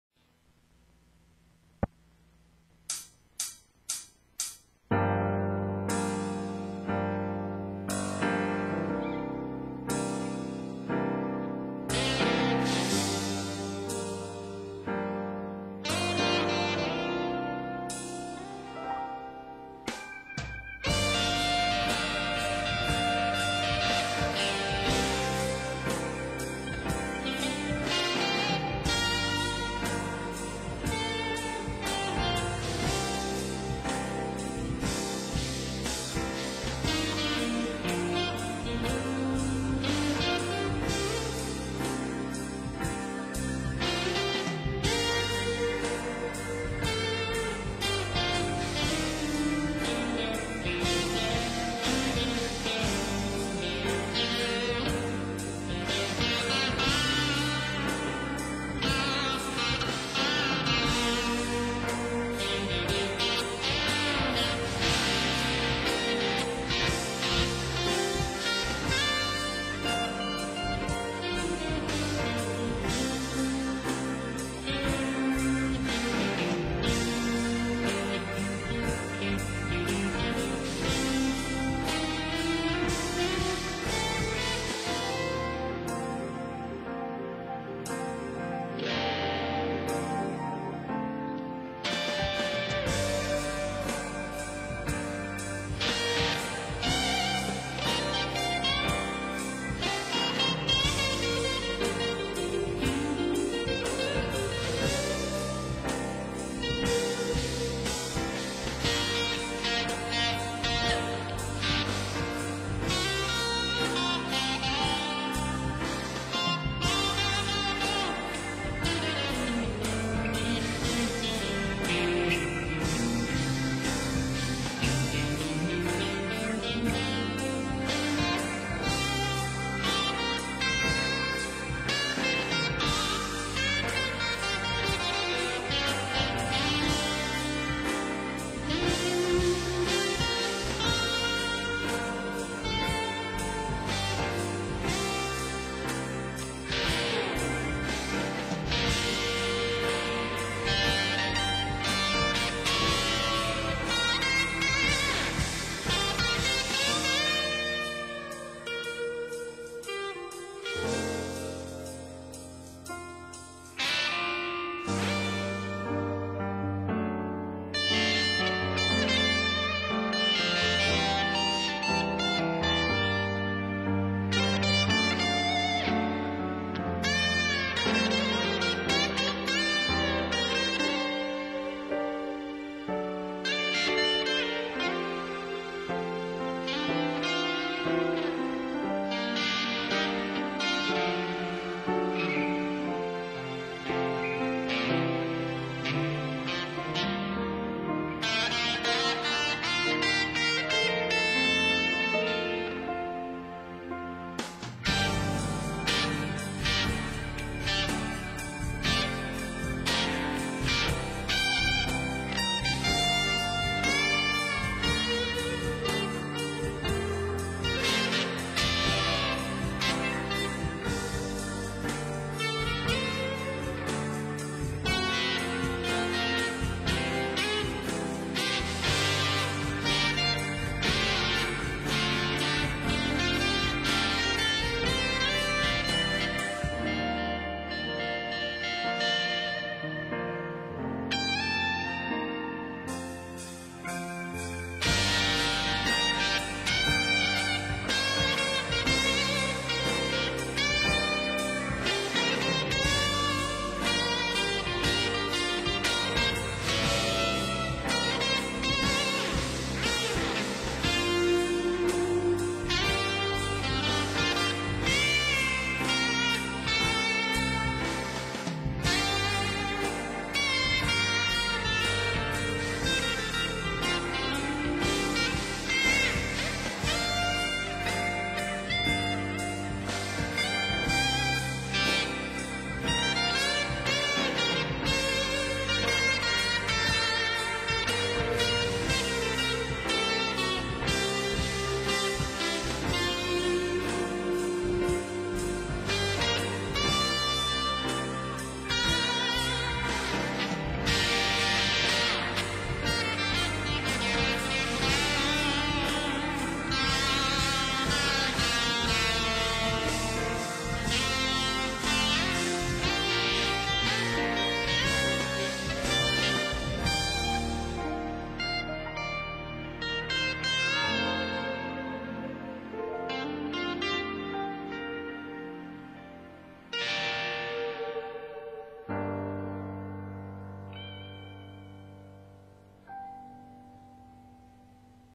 floydish jam.wmv
my responce to jam .. sorry for bad playing was in a mood just wanted to post something and jam ,